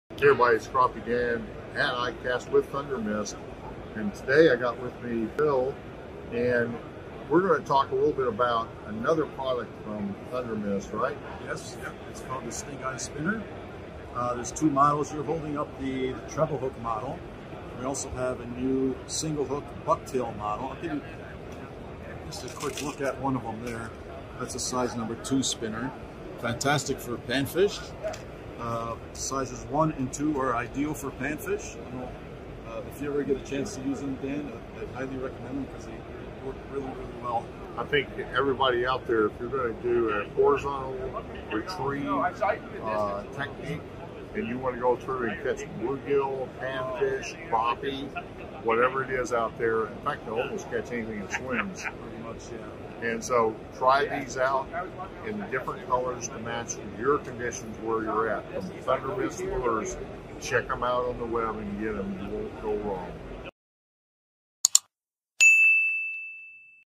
Your New Favorite Spinner?
Throwback to 2023 ICAST where Crappie Dan met up with Phil from Thundermist to discuss their Stingeye Spinner and how it can help YOU catch more crappie!